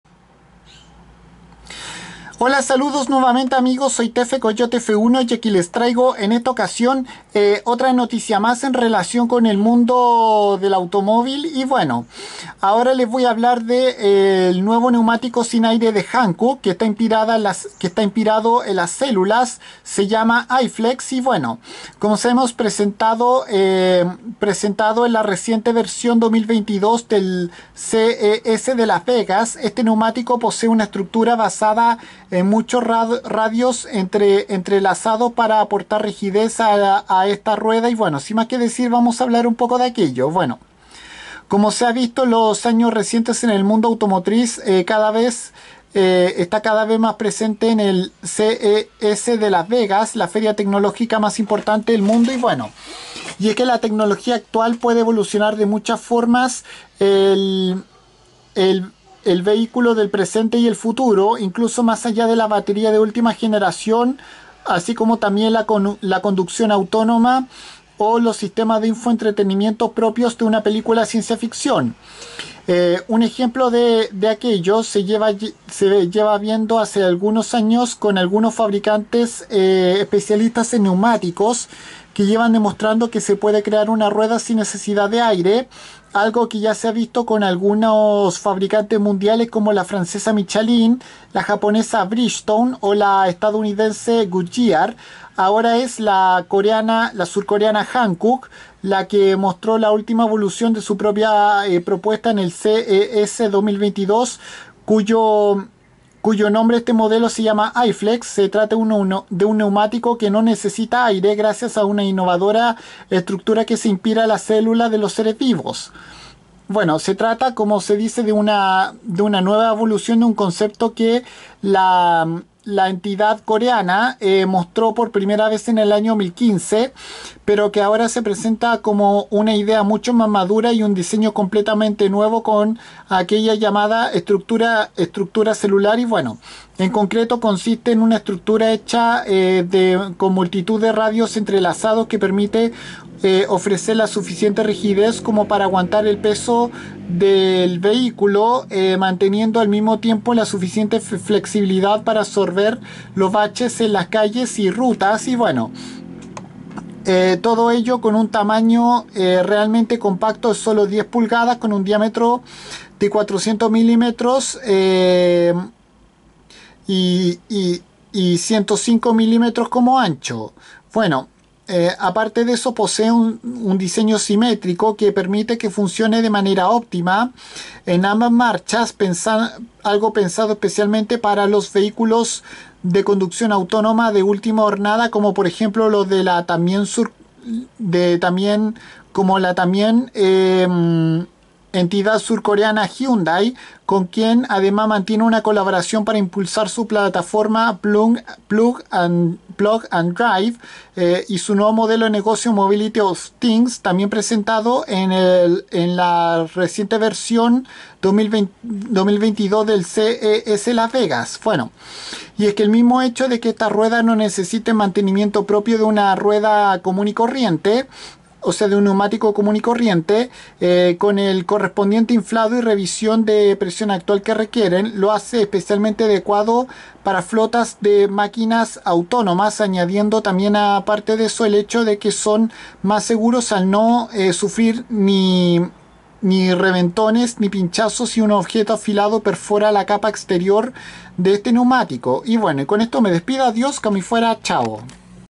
Neumático Hankook i-Flex información
En este vídeo les comento sobre como es este novedoso neumático que no necesita aire presentado en la reciente versión 2022 del CES de Las Vegas por la entidad surcoreana y que está inspirado en las células de los seres vivos